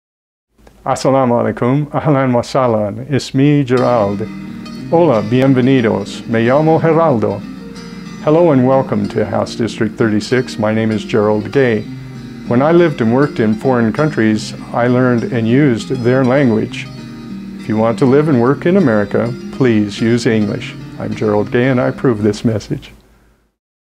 Gerald Gay commercial - PLEASE SPEAK ENGLISH 2.wmv
When I lived and worked in other countries, I learned and used their language. If you want to live and work in America, please learn and use English.